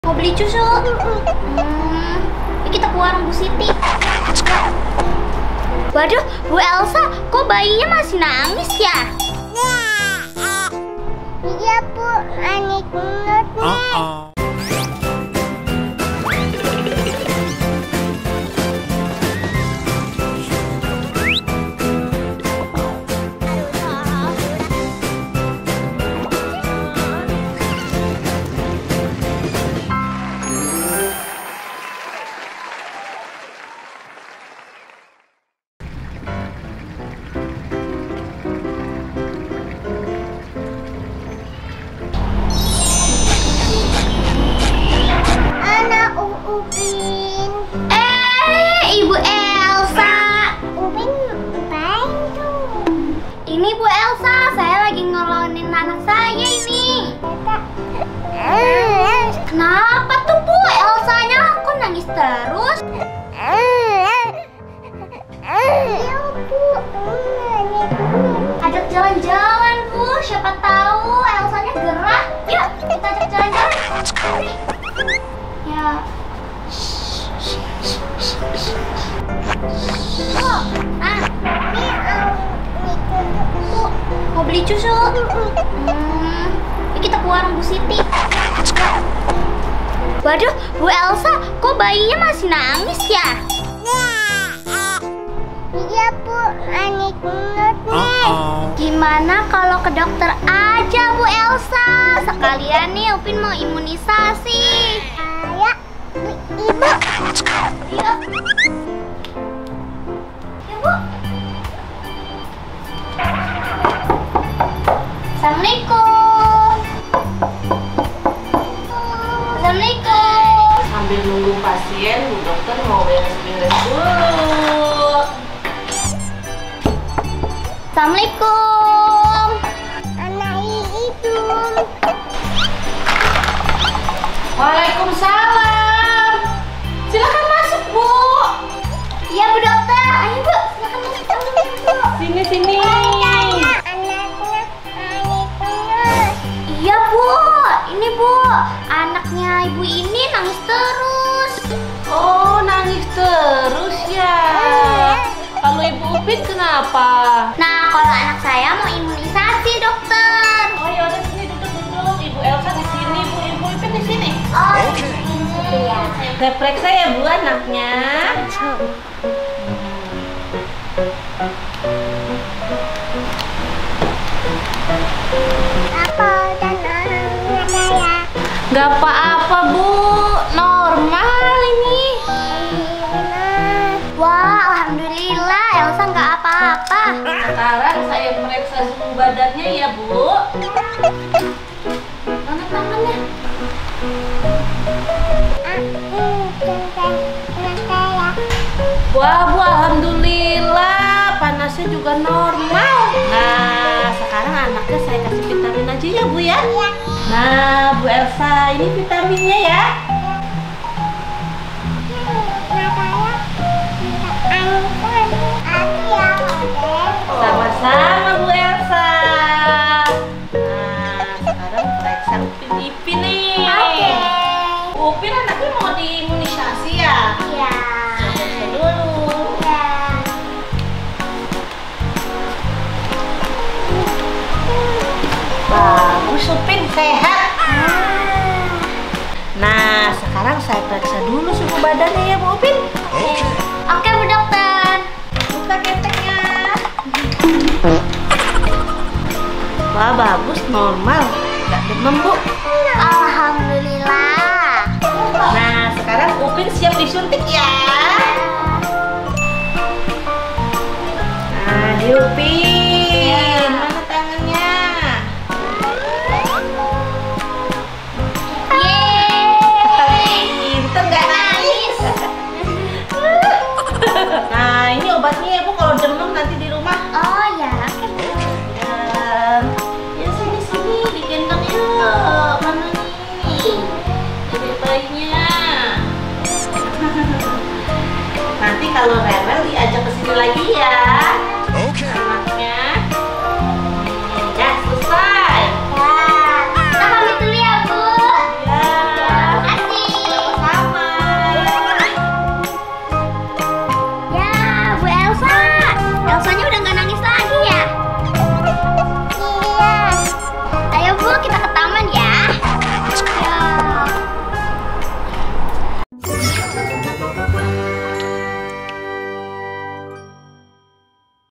MAIN BAYI BAYIAN _ 2 BAYI ESLA DAN UPIN PERIKSA KE DOKTER
Teman teman di video kali ini Fakhira dan Arumi bermain Bayo bayian , eh bayi Elsanya nangis terus akhirnya dibawa kedokter dan bayi Upin imunisasi ..
Semoga Terhibur..